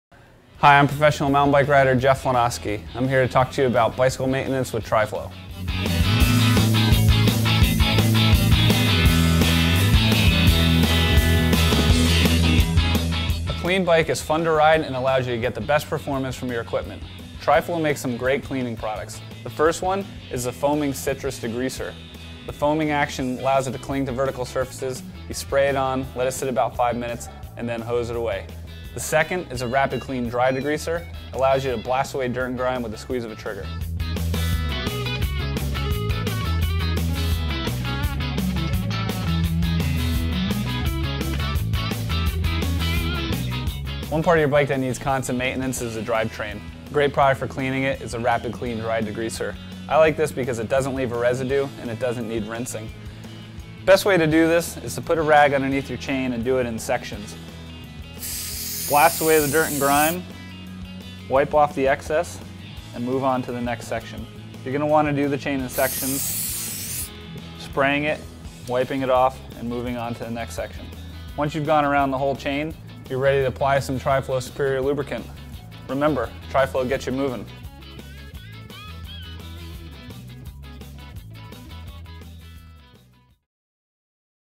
Tri-Flow Degreaser How To Video with Jeff Lenosky
Tri-Flow How To Video with Jeff Lenosky highlighting Tri-Flow Rapid Clean and Tri-Flow Citrus Degreaser. Tri-Flow Gets It Movin'.